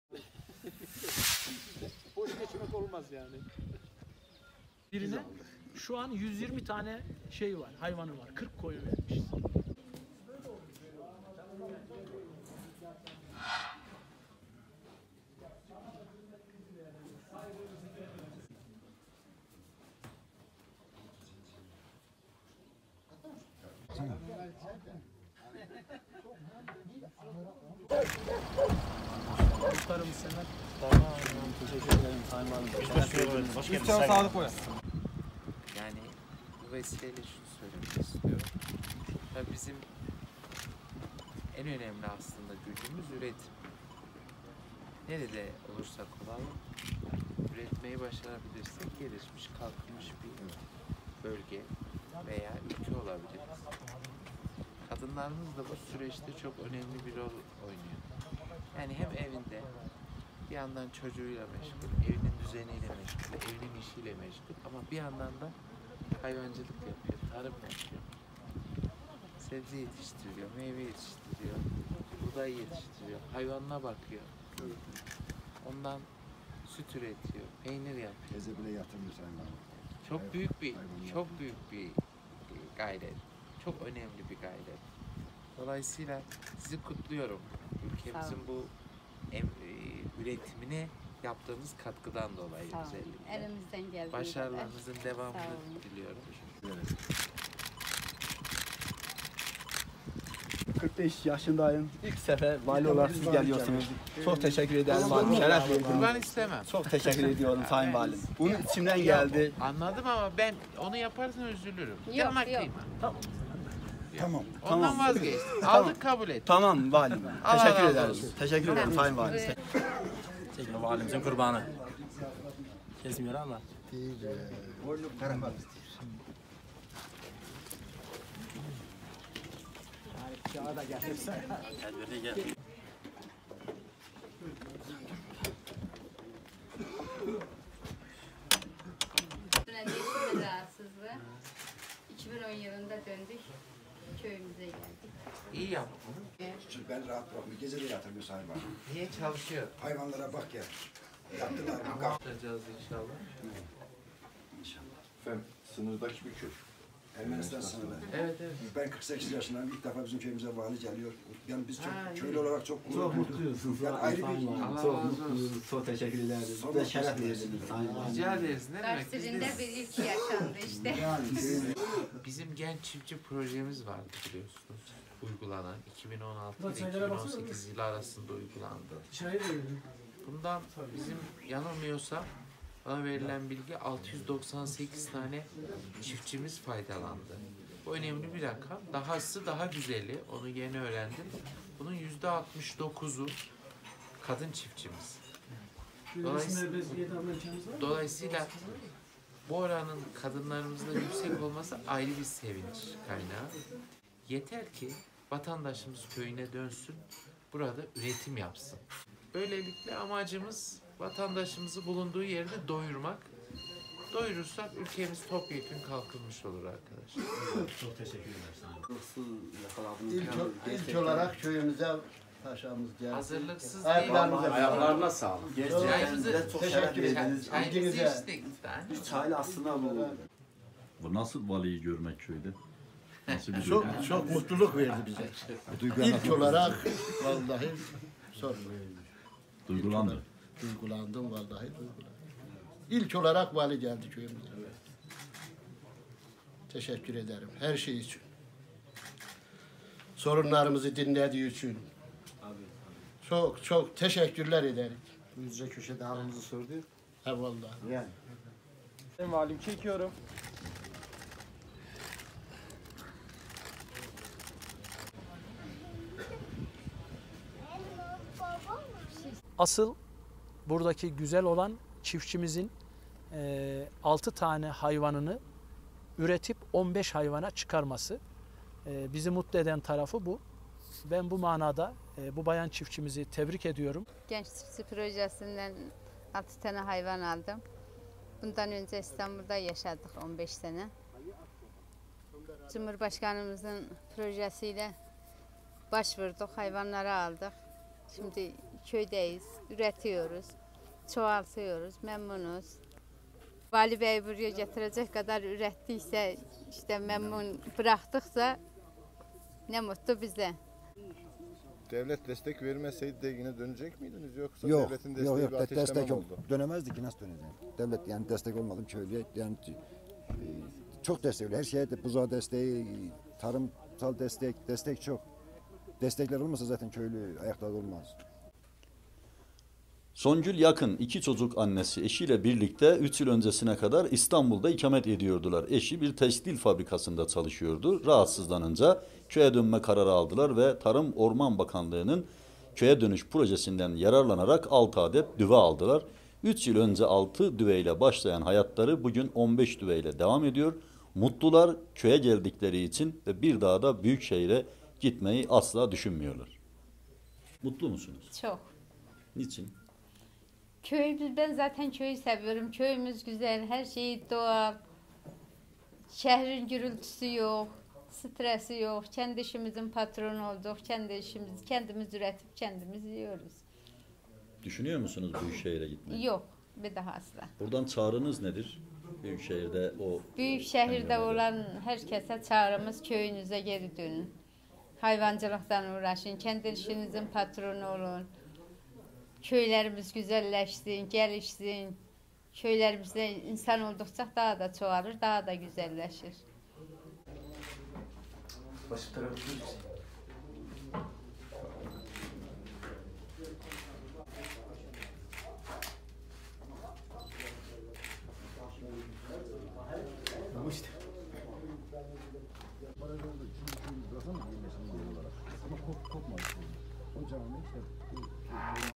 Kars’ta köye ilk ayak basan Valiye bir kadının başarısı sebep oldu
Kars’ın merkeze bağlı Ermenistan sınırındaki Yalçınlar Köyü’ne tarihinde ilk kez bir vali ayak bastı. Tarım ve Orman Bakanlığı’nın Köye Dönüş Projesi kapsamında hayata geçirdiği Genç Çiftçiler Projesi kapsamında 3 yıl önce İstanbul’dan köyüne dönen Songül Yakın’ın başarılı çalışmaları ve üretime katkısı Vali Türker Öksüz’ü köye götüren sebep oldu.